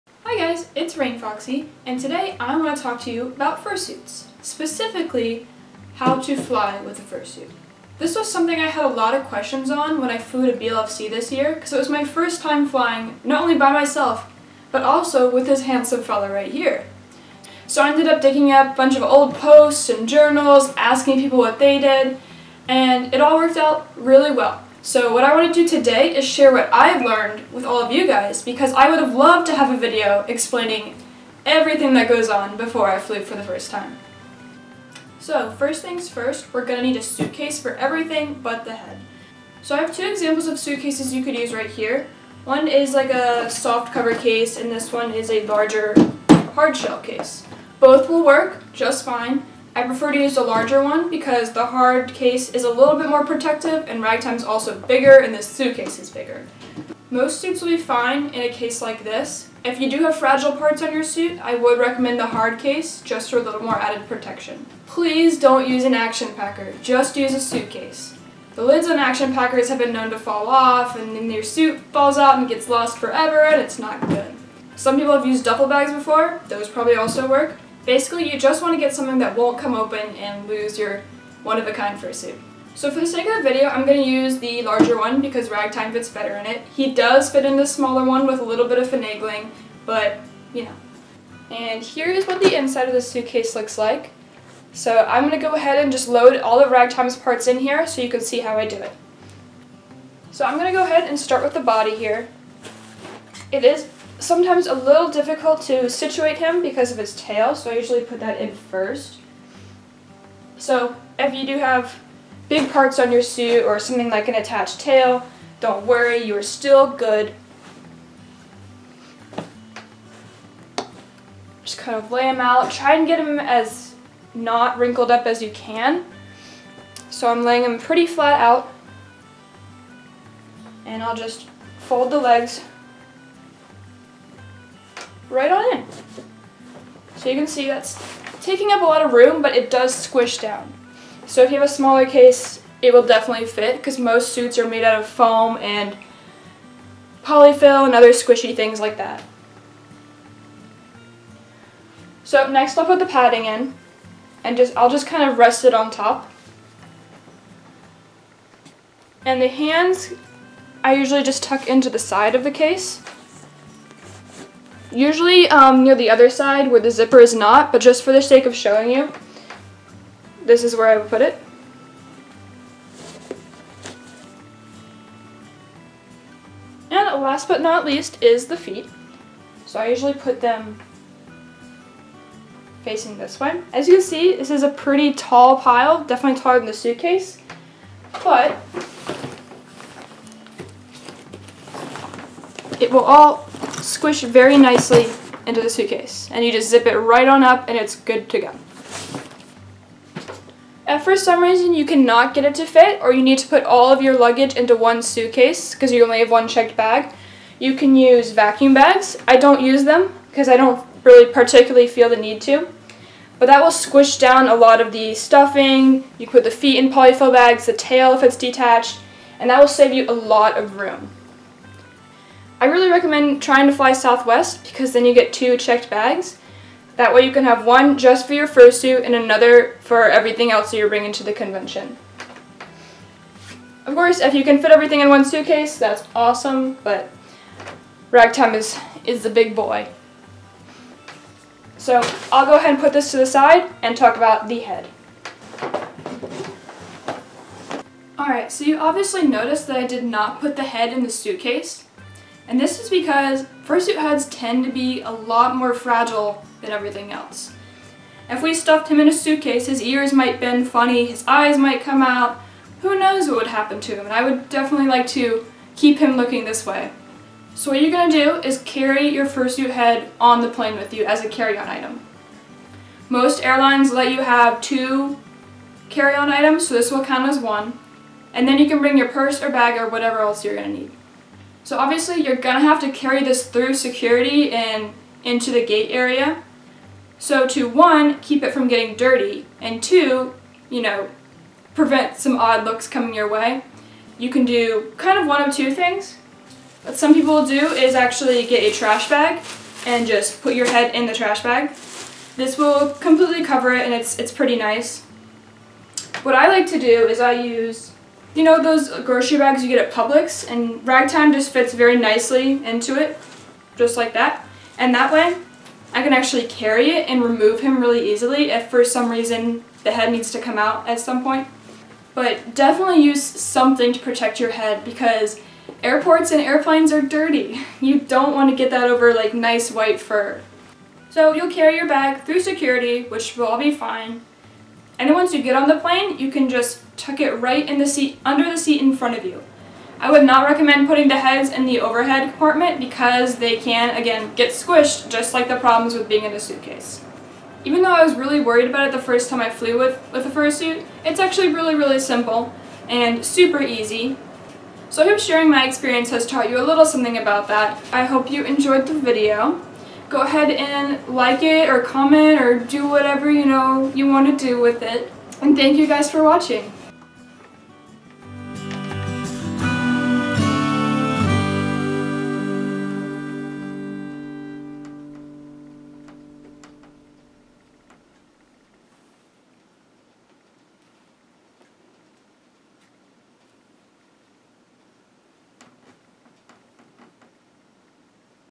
How to Fly with a Fursuit
Short little (hopefully) informative video! Like explained in the video, I was really nervous about flying with Ragtime for the first time to BLFC, and I wish there had been a video like this I could have watched! I briefly go over a couple things you'll want to do.

*NOTE: Wait to pack your suit until the night before you leave for your flight. You want to leave it squished in there for the shortest time possible, especially if you are using vacuum bags. Forgot to say that :)